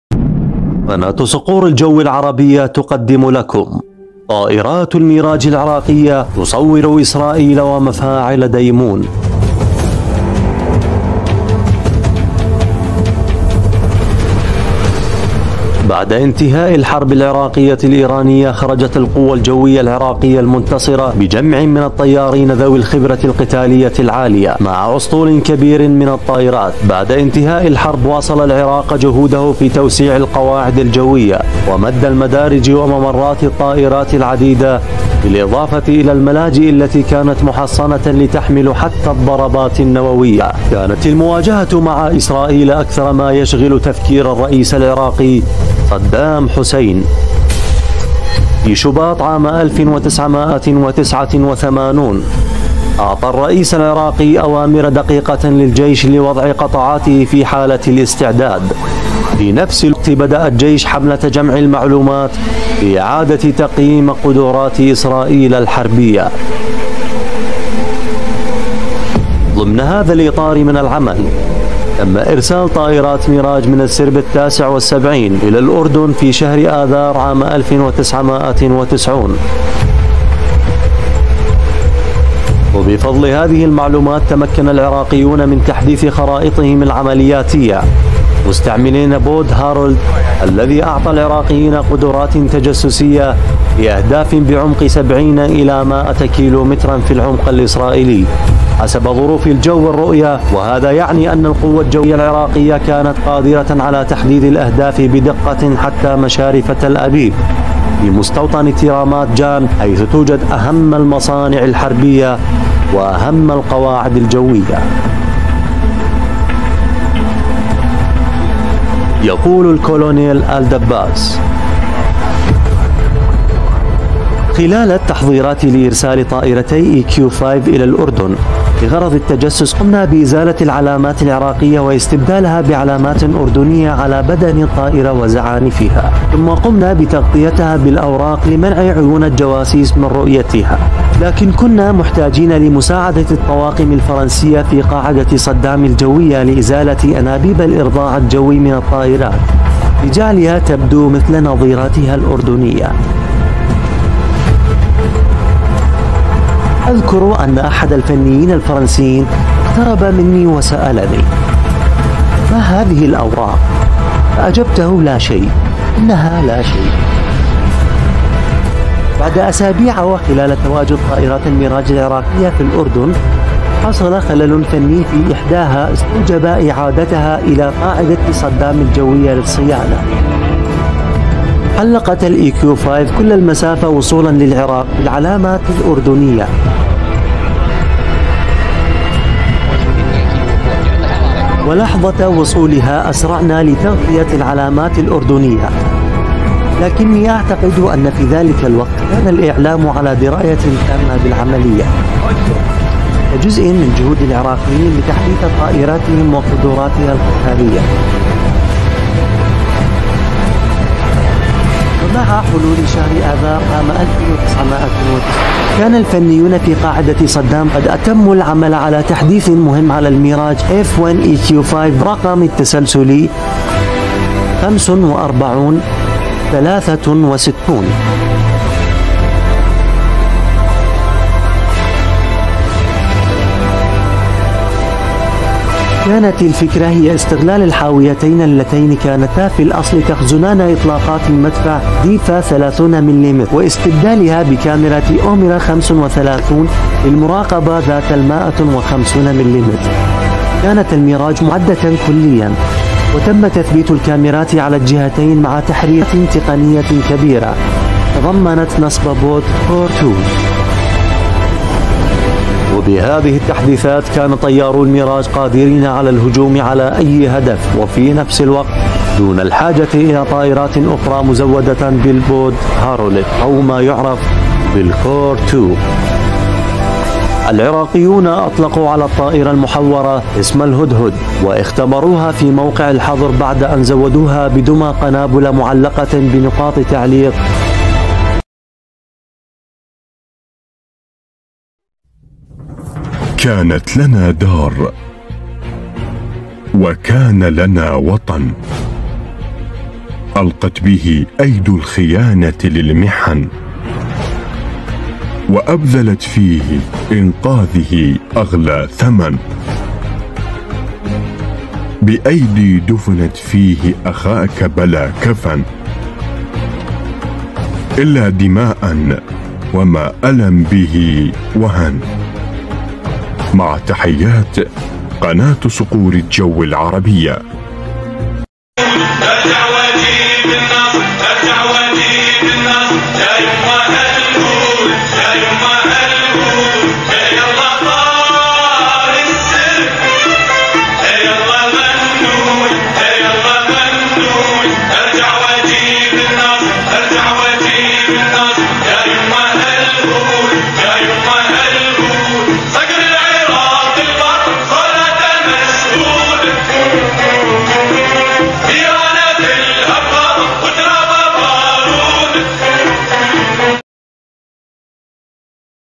طائرات الميراج العراقية تصور إسرائيل ومفاعل ديمونة - اكتشف الحقيقة!
طائرات الميراج العراقية تسجّل إسرائيل ومفاعل ديمونة - اكتشف الحقيقة!  @Suqour.Aljaw.Alarabiyah ​
في هذا الفيديو الحصري، ستشاهد لقطات مذهلة تم تصويرها بواسطة طائرات الميراج العراقية وهي تسجّل إسرائيل ومفاعل ديمونة. هذه اللقطات تكشف النقاب عن قصة مثيرة وغامضة للتجسس الجوي والتحقيق الخطير. انضم إلينا لاستكشاف الحقيقة وراء هذه الصور النادرة والمشوقة.
التوتر مع إسرائيل
بعد انتهاء الحرب العراقية الايرانية خرجت القوة الجوية العراقية المنتصرة بجمع من الطيارين ذوي الخبرة القتالية العالية مع اسطول كبير من الطائرات
بعد انتهاء الحرب واصل العراق جهوده في توسيع القواعد الجوية و مد المدارج و ممرات الطائرات العديدة بالاضافة الى الملاجئ التي كانت محصنة لتحمل حتى الضربات النووية
كانت المواجهة مع إسرائيل اكثر ما يشغل تفكير الرئيس العراقي صدام حسين
في شباط ١٩٨٩ اعطى الرئيس العراقي اوامر دقيقة للجيش لوضع قطعاته في حالة الاستعداد، في نفس الوقت بدأ الجيش حملة جمع المعلومات لإعادة تقييم قدرات إسرائيل الحربية
ضمن هذا الإطار من العمل تم إرسال طائرات ميراج من السرب التاسع والسبعين الى الاردن في شهر اذار ١٩٩٠
من مطار الزرقاء قامت الميراج بطلعات جوية على طول نهر الاردن لجمع المعلومات من العمق الاسرائيلي
و بفضل هذه المهمات تمكن العراقيون من تحديث خرائطهم العملياتية مستعملين بود هارولد الذي اعطى العراقيين قدرات تجسسية لأهداف بعمق ٧٠ الى ١٠٠ كيلومتر في العمق الاسرائيلي حسب ظروف الجو و الرؤية و هذا يعني ان القوة الجوية العراقية كانت قادرة على تحديد الاهداف بدقة حتى مشارف تل ابيب في مستوطنة رامات جان حيث توجد اهم المصانع الحربية و اهم القواعد الجوية
طائرات الميراج العراقية، تصوير، إسرائيل، مفاعل ديمونة، التجسس الجوي، الصور النادرة
@Suqour highlights aljazeera #tiktok tiktoklongtho يوتيوب ط الجزائر يار